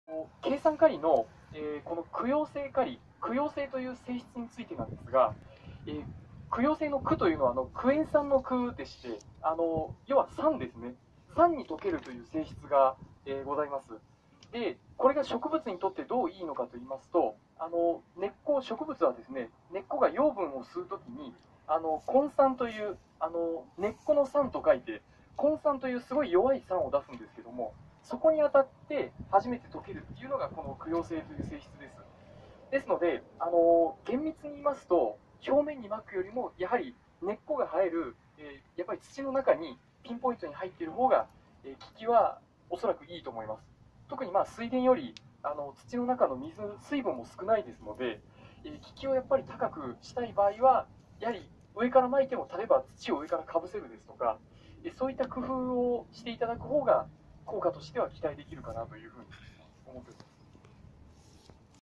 20180620果樹研究会1453〜フェザー苗木現地検討会、珪酸カリプレミアム34
施肥方法
水に溶けないので、効果持続時間が長い